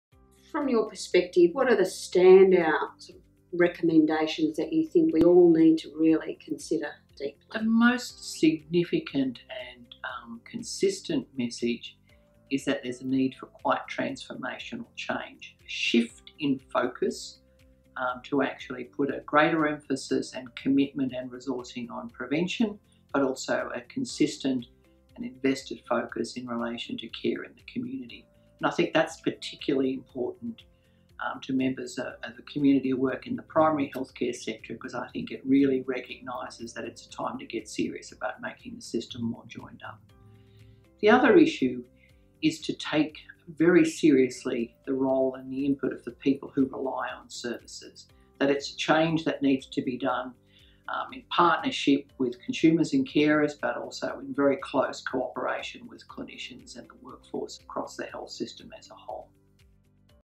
Part 2/3 - Better health, together: Sustainable Health Review
"What are the key findings within the Final Report?"

In this month's Better, health, together video, WA Primary Health Alliance CEO, Learne Durrington is joined by Sustainable Health Review Chair, Robyn Kruk to talk about the WA Department of Health's recently released Sustainable Health Review Final Report.